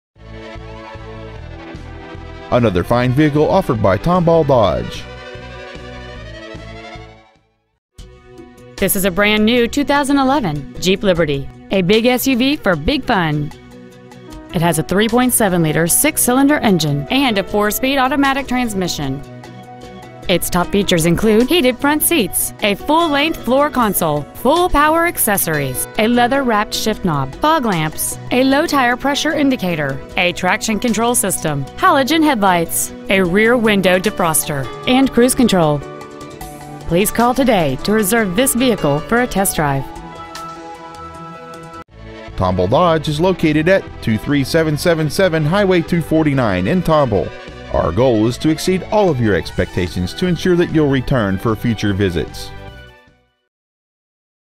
2011 Jeep Liberty Conroe TX
Year : 2011

 Make : Jeep

 Model : Liberty

 Engine : 3.7L V6 ENGINE

 Trans . : Automatic

 Exterior : Deep Cherry Red Crystal Pearl

 Miles : 44

 Tomball Dodge

 23777 HWY 249

 2011 Jeep Liberty  TX